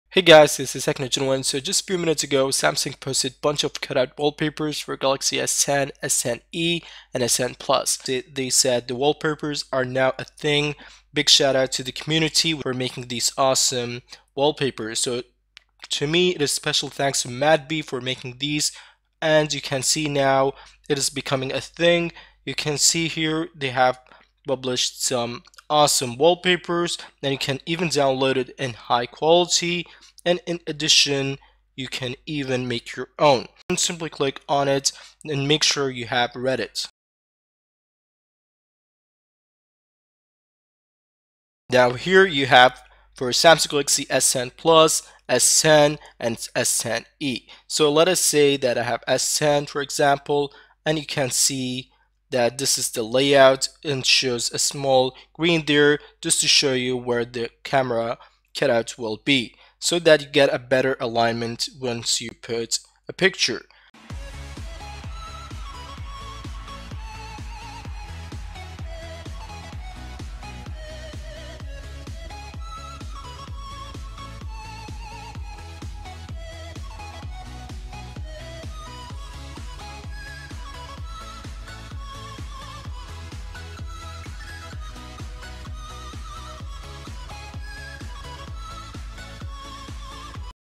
Official Samsung Galaxy S10 Cutout Wallpapers | Hide Cutout
Official Cutout Wallpapers from Samsung. You can even make one for your own using the template suggested by Samsung.